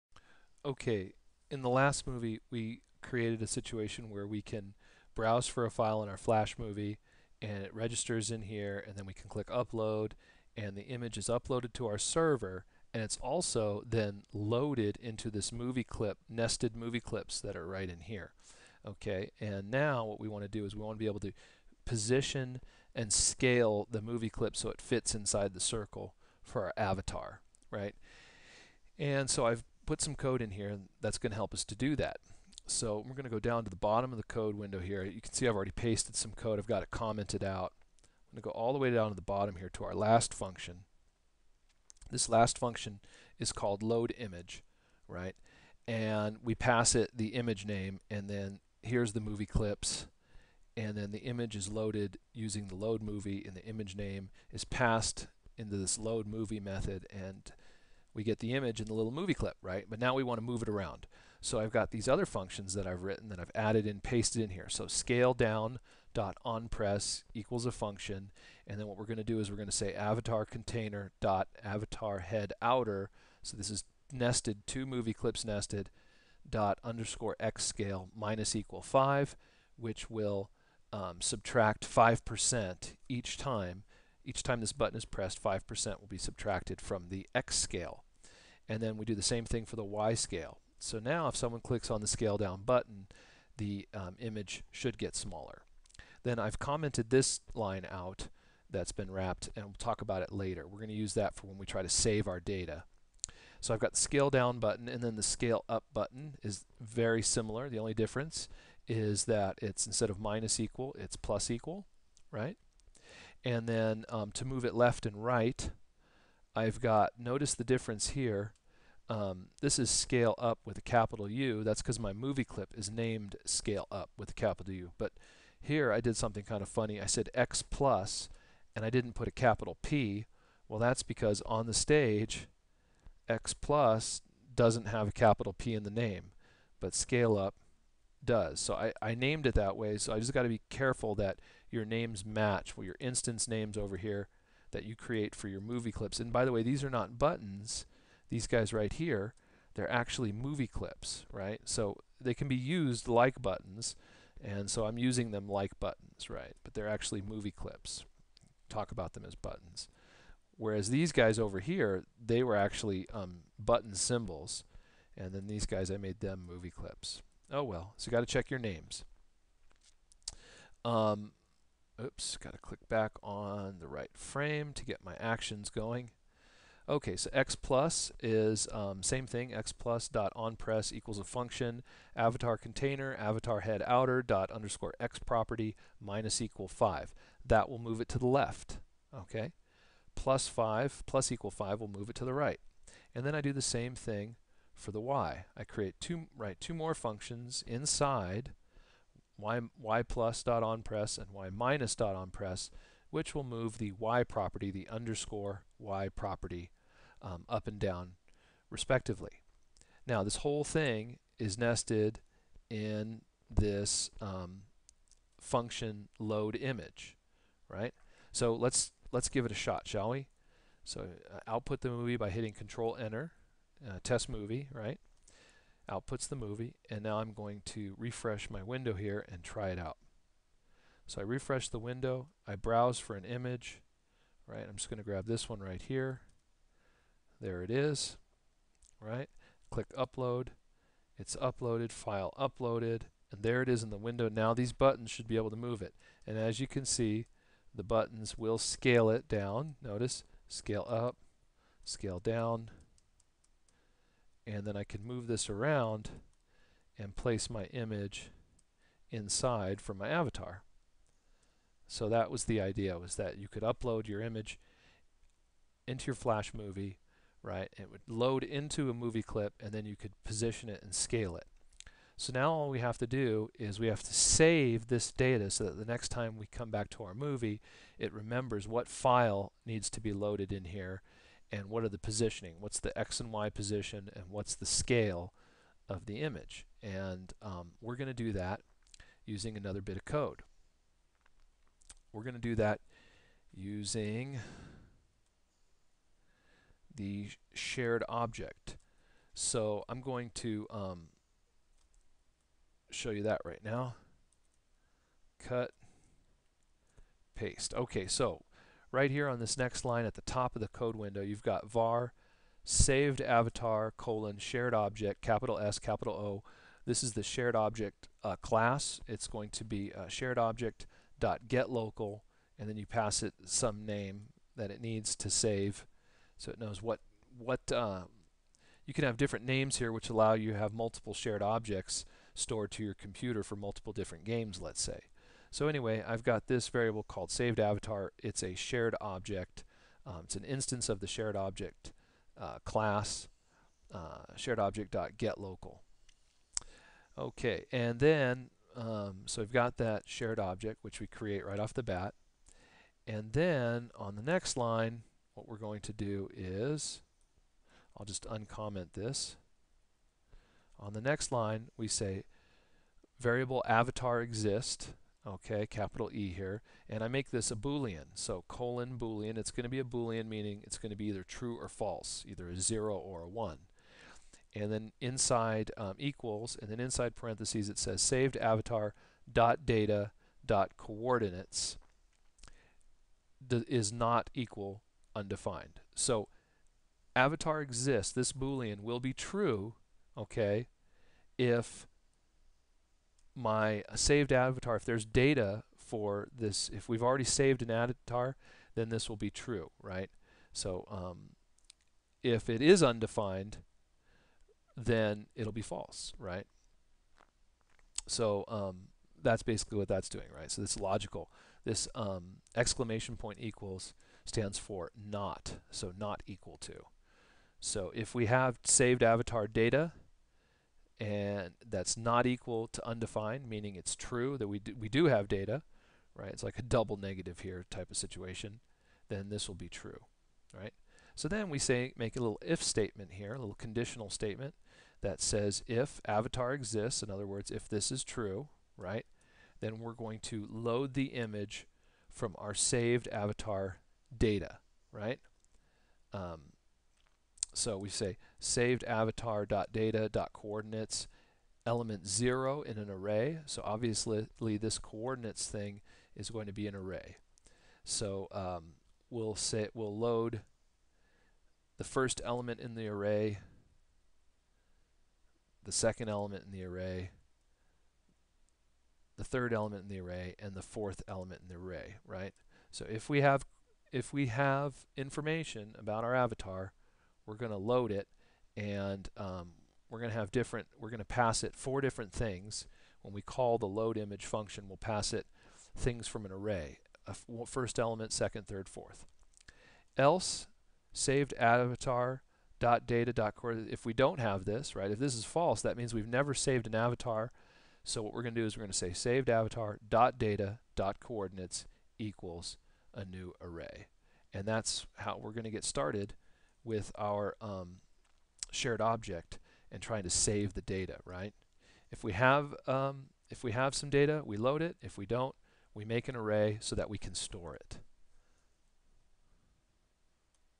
Upload Images and Saving the Data Using Flash - Part 5 (Actionscript 2.0)
A tutorial on using the Flash FileReference Class and the SharedObject Class to upload image files to your server using Flash Actionscript 2.0 and save the Data to your local computer. Upload your own chatroom avatar and save your settings.